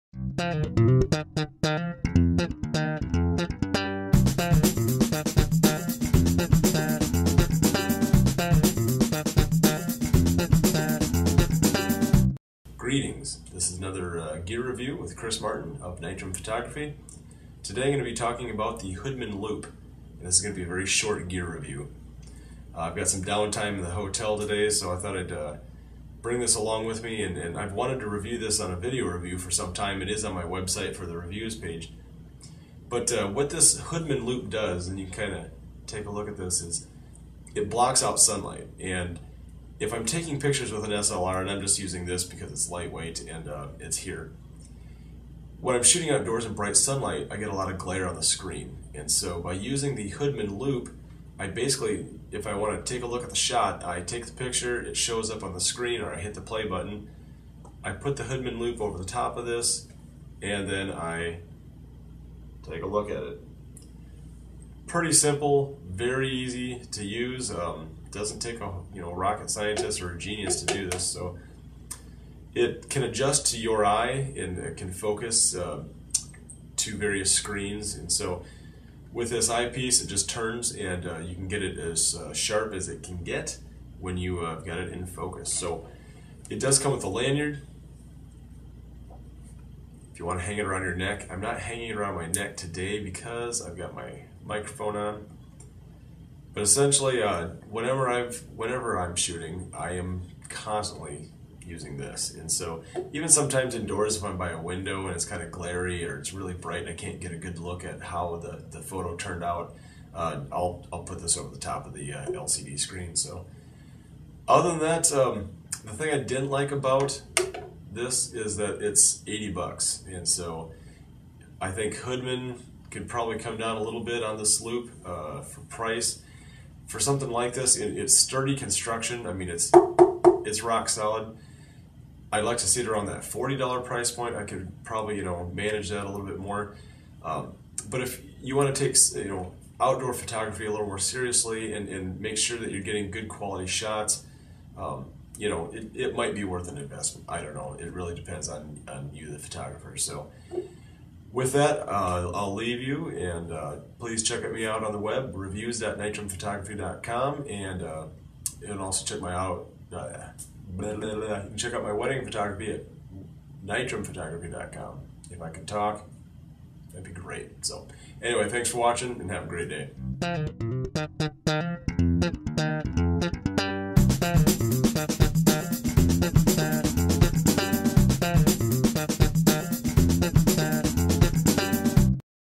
Hoodman Loupe
A quick review of what you might use a Loupe for when shooting outdoors.